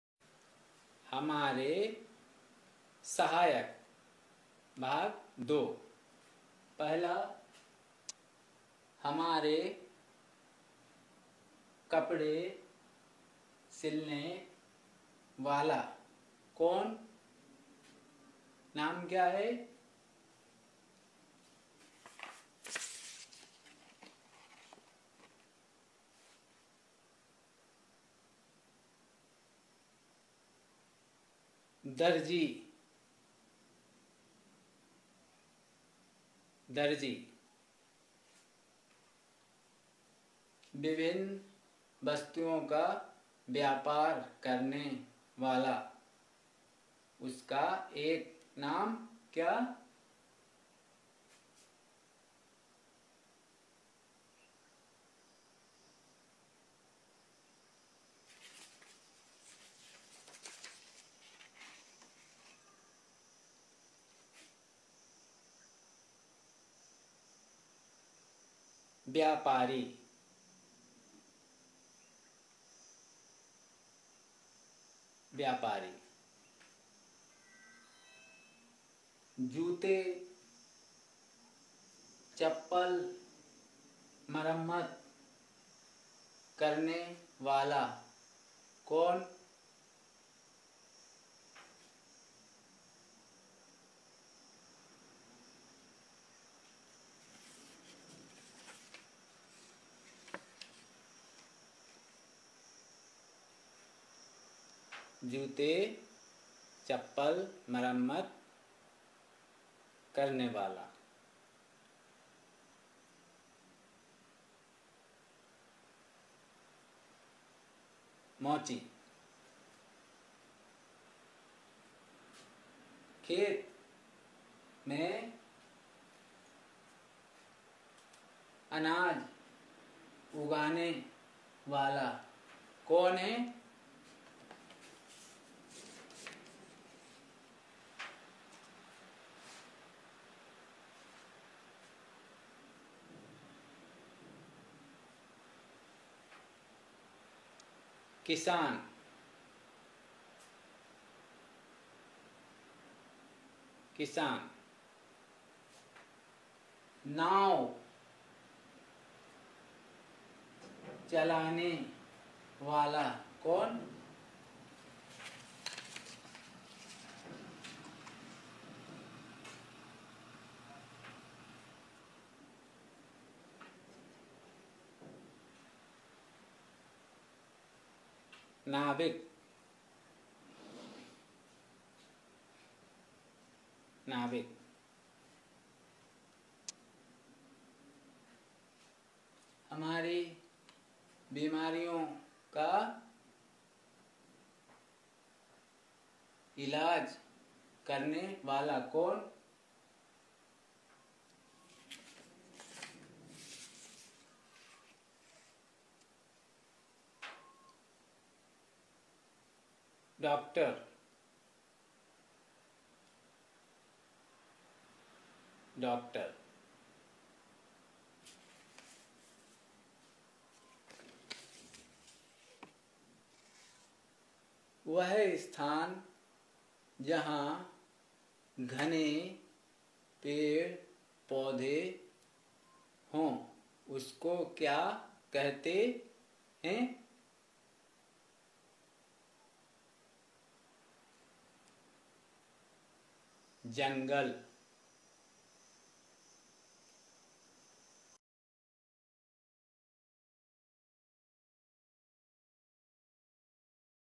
वाक्यांश के लिए एक शब्द सांकेतिक भाषा | हमारे सहायक | One word for phrase our helper sign language
इस वीडियो के बारे में/About this video -
इस वीडियो के माध्यम से हमारे सहायक (कार्य और व्यवसाय- our helpers ,work and profession) के माध्यम से वाक्यांश के लिए एक शब्द और उन्हें सांकेतिक भाषा में व्यक्त करना सिखाना ।